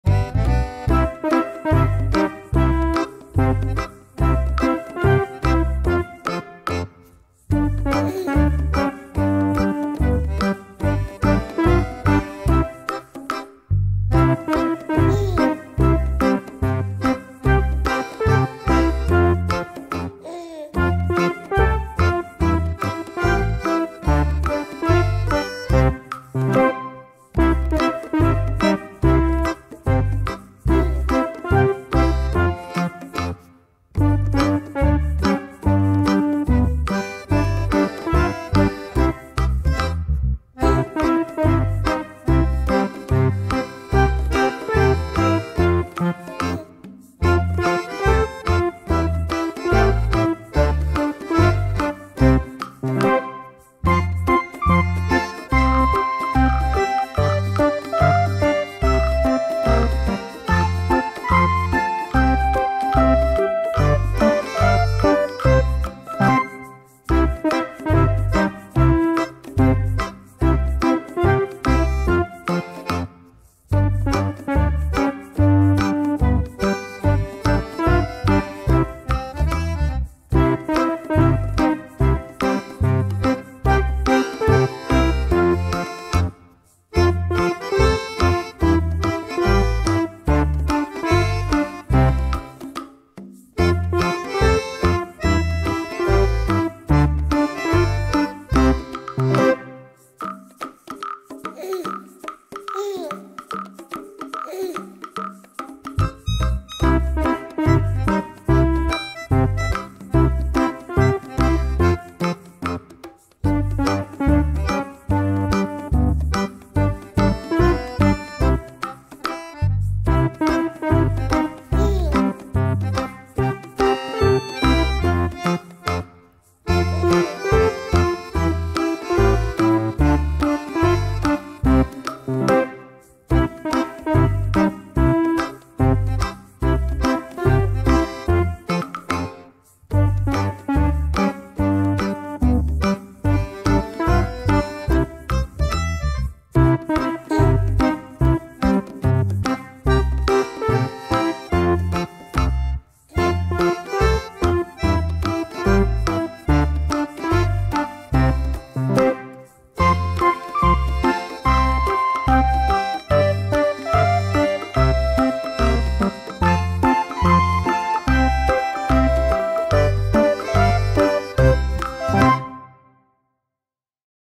Drawing Colorful owls, Painting, Coloring Toddlers & Kids | How to draw easy drawings #2023
owl is one of my favorite toys. Do you like playing with toy cranes? Let's learn to draw, paint easy pictures together...

10:00 p.m